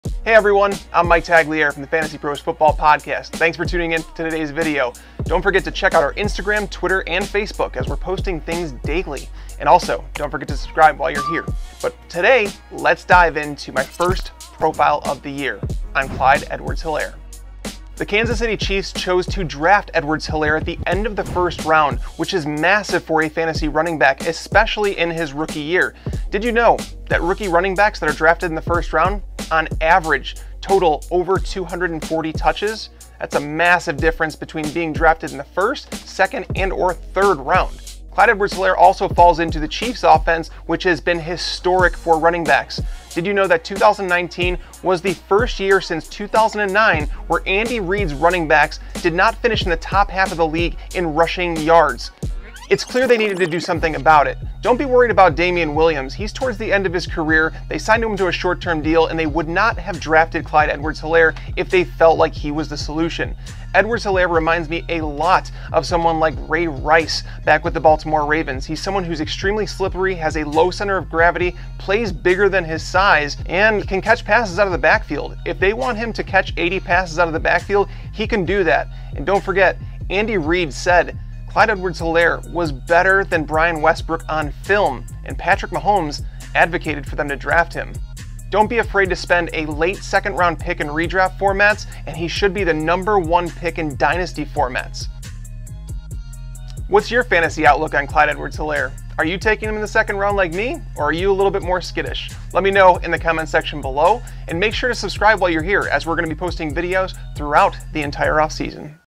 Clyde Edwards-Helaire Highlights + Fantasy Predictions (2020 Fantasy Football)
Clyde Edwards-Helaire is a true three-down back who can catch 80-plus passes out of the backfield, while running for 1,000-plus yards. Landing with the Chiefs was a dream scenario, especially in the first round. Over the last seven years, running backs who've been drafted in the first round have averaged 280.7 touches in year one. 75 percent of them have finished as an RB2 or better. Don't think this is Darwin Thompson all over again, as he was a sixth-round pick last year, and those average just 29.8 touches in year one. This is Edwards-Helaire's backfield after Andy Reid said he's better than Brian Westbrook on film. You don't take those players off the field very often.